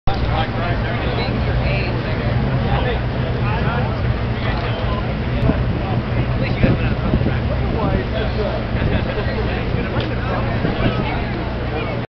Graham Rahal & His Girlfriend at the Honda Indy Toronto video 3 of 9
Indycar driver Graham Rahal and his girlfriend after practice at the Honda Indy Toronto July 2010 walking from the track back to their paddock.